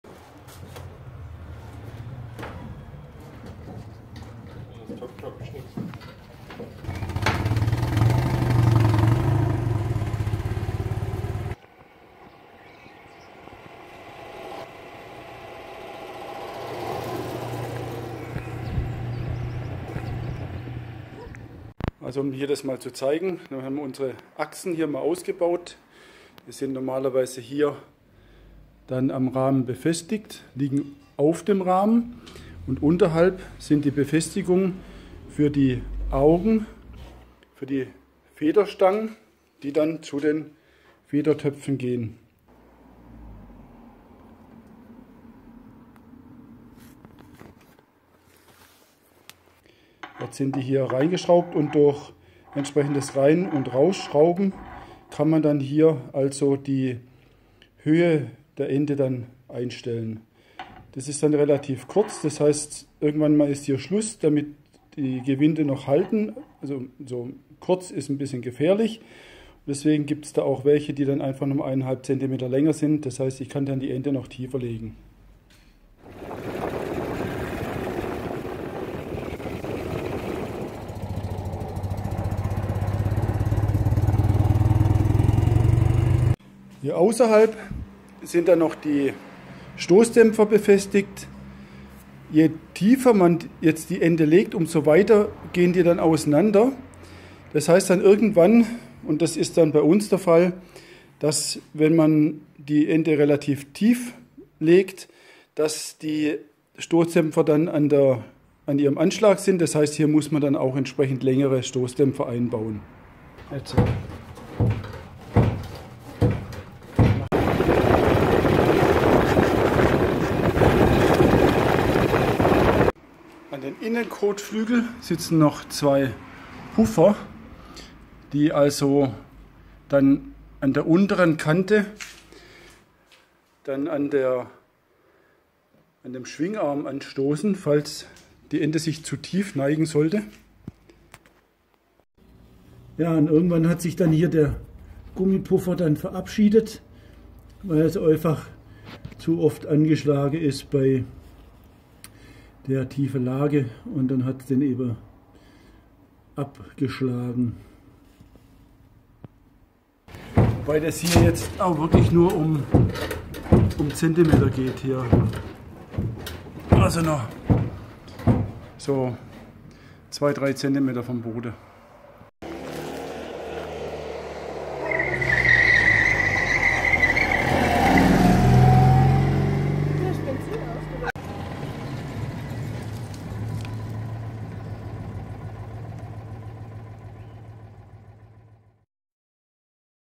Ente tiefer legen - So haben wir das gemacht
Wir haben uns das angesehen, was passiert, wenn man die Ente tieferlegt. Neben Verlängerung der Federzugstrebe müssen auch die Stoßdämpfer ausgetauscht werden. Achtung, beachtet was gesetzlich erlaubt ist.